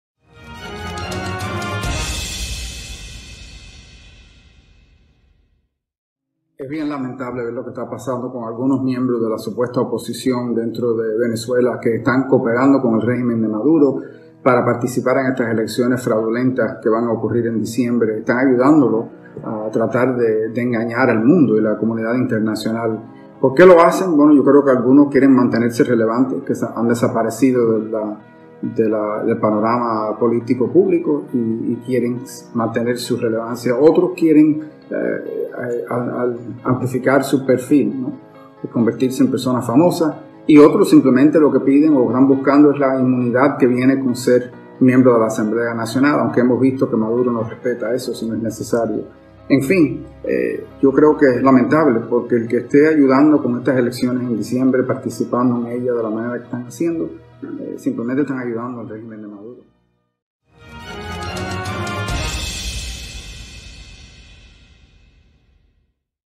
"ES BIEN LAMENTABLE VER LO QUE ESTA PASANDO CON ALGUNOS MIEMBROS DE LA SUPUESTA OPOSICIÓN"
El senador republicano @marcorubio, se pronunció sobre la decisión de algunos dirigentes de oposición de participar en las venideras elecciones parlamentarias. “Creo que algunos quieren mantenerse relevantes porque han desaparecido del panorama político”,